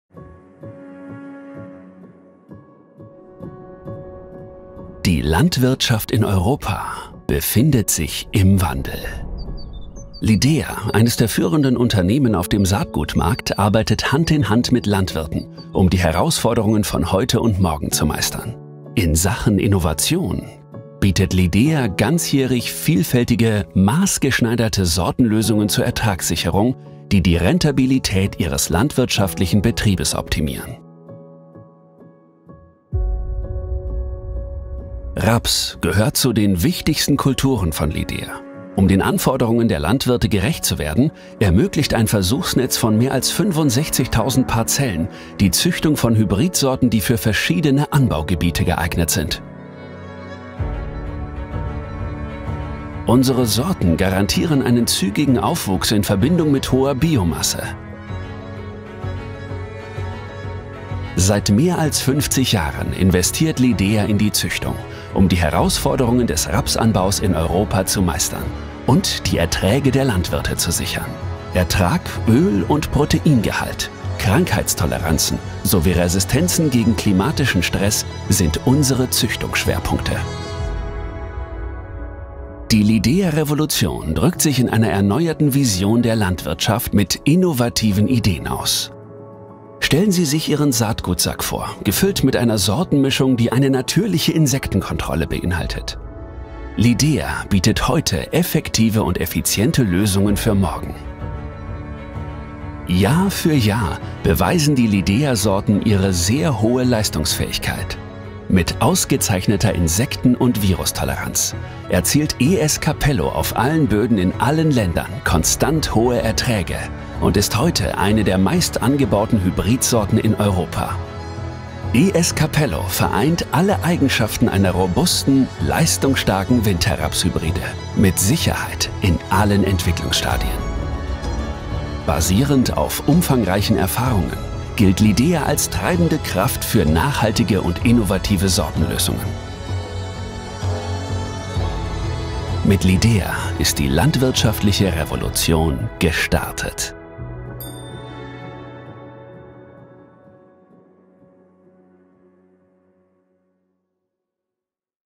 Focus auf die anstehende Rapssaison 🐝
Seit mehr als 50 Jahren investiert LIDEA in die Züchtung, um das genetische Potenzial von Winterraps bestmöglich auszuschöpfen. Ziel ist es, den Landwirten robuste, leistungsstarke Sorten mit hoher Qualität zu liefern.